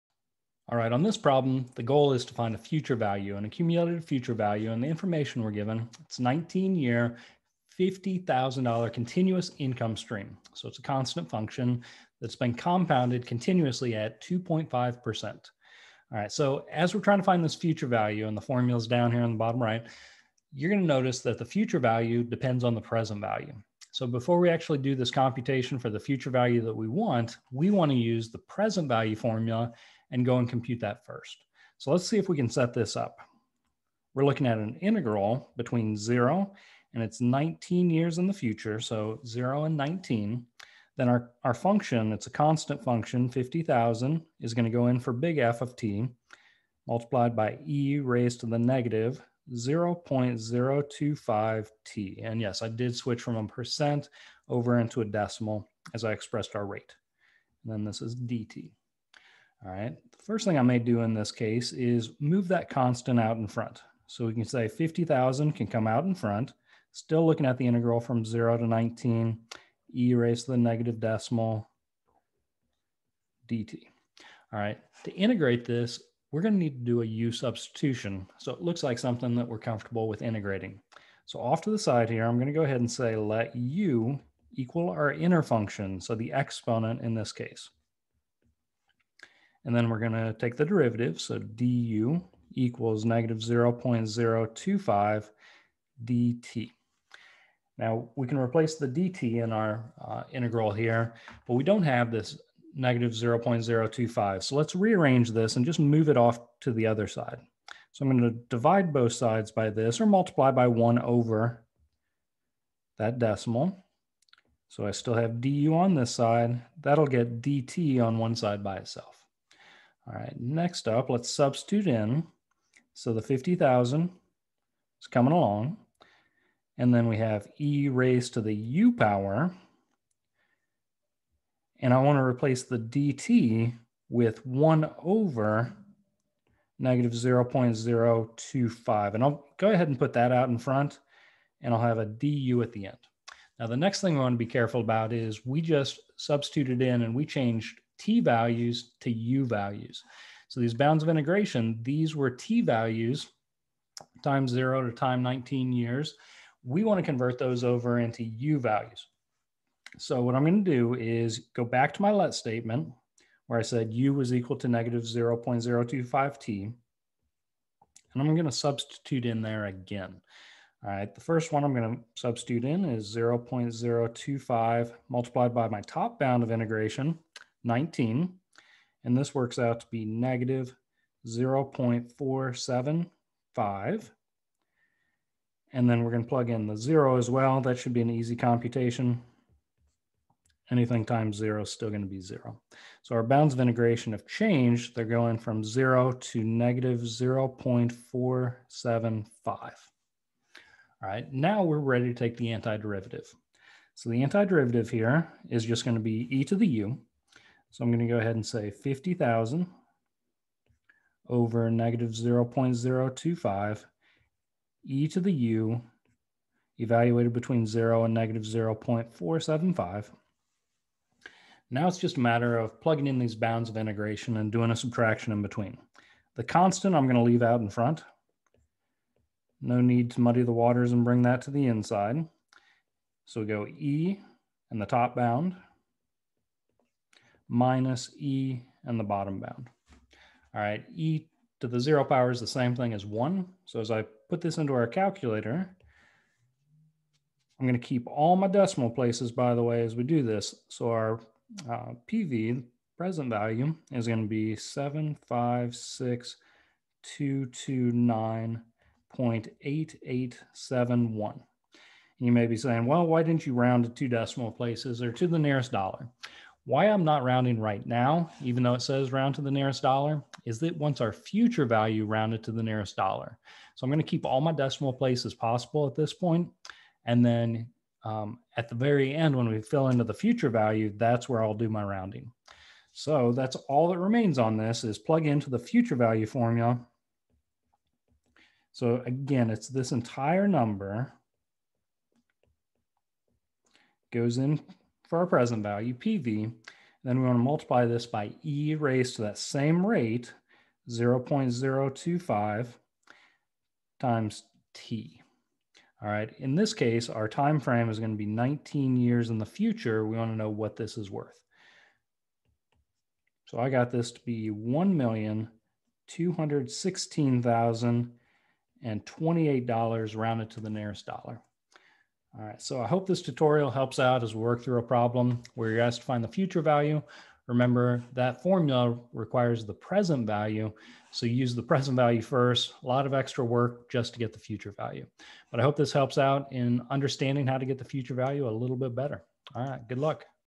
Future value of a constant continuous income stream present value business calculus
In this calculus example, we find the future value when given a time frame, constant continuous income stream, and rate.  To do so, we must first find the present value by filling into the present value formula and integrating.  In doing so, we must use u substitution and be careful to adjust our bounds of integration.  After we have found the present value, we fill into the future value formula and calculate what it will be worth in the future.